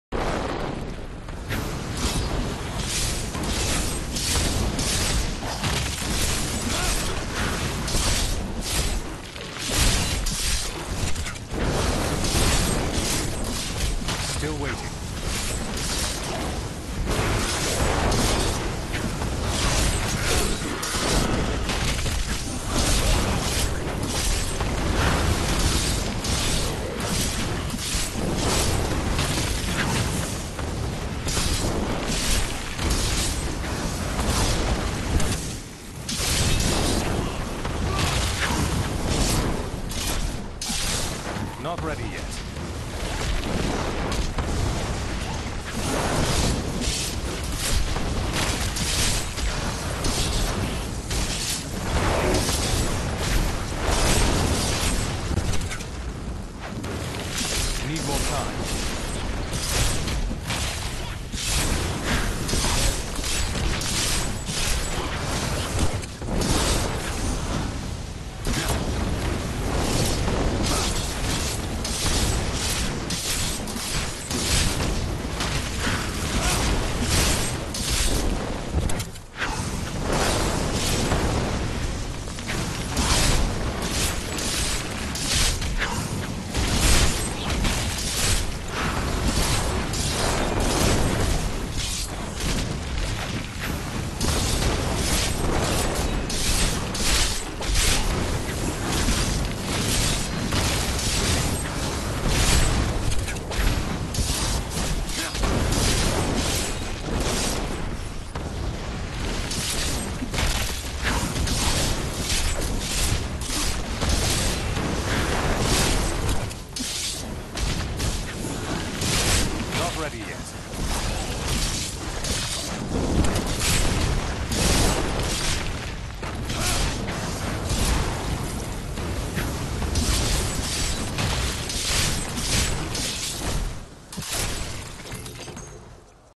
Lacuni Stalkers - Arcane Enchanted, Desecrator, Shielding, Waller. (1.0.3)
Critical mass wizard act II showcase.

7700 Armor, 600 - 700 res all, 22K dps, 300 LoH